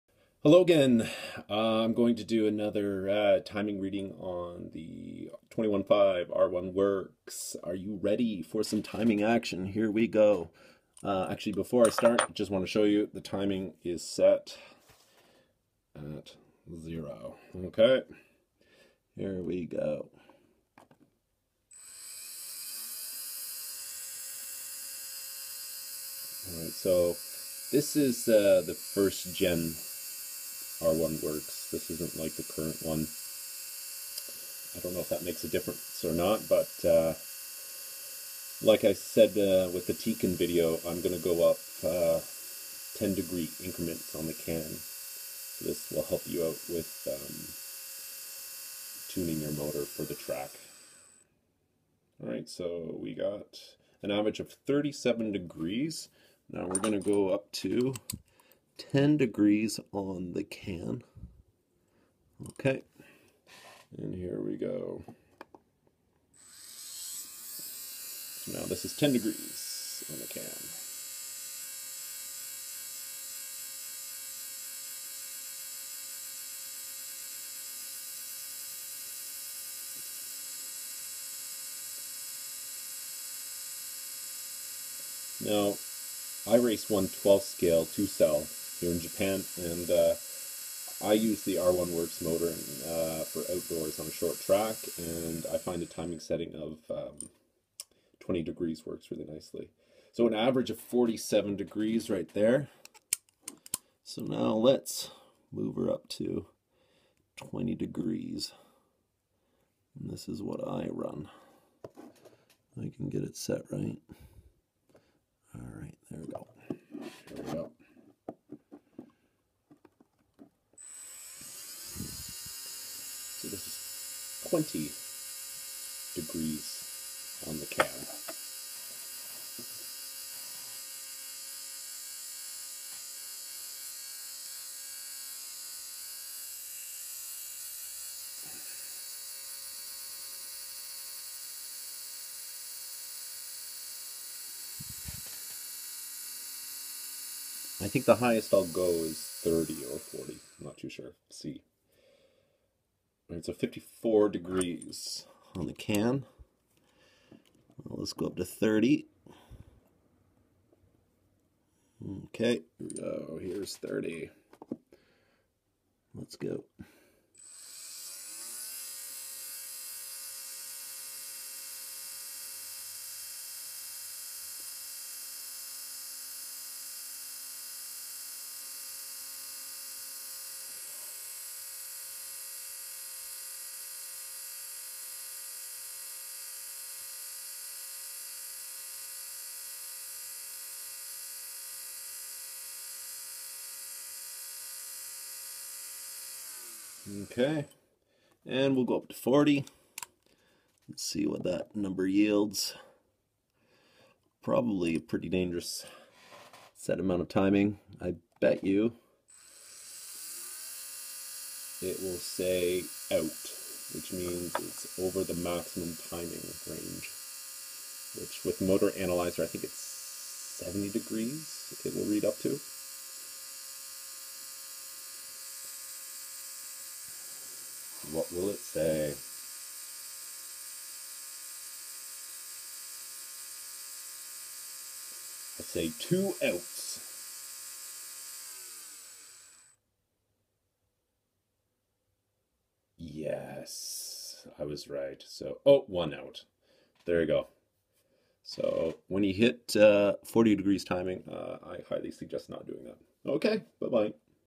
R1 Wurks 21.5 Timing Settings
Put the R1 Wurks on the motor analyzer to give you the exact timing readings the motor gives you. Remember the timing settings on my motor may not necessarily be the same on yours. I hope this video helps.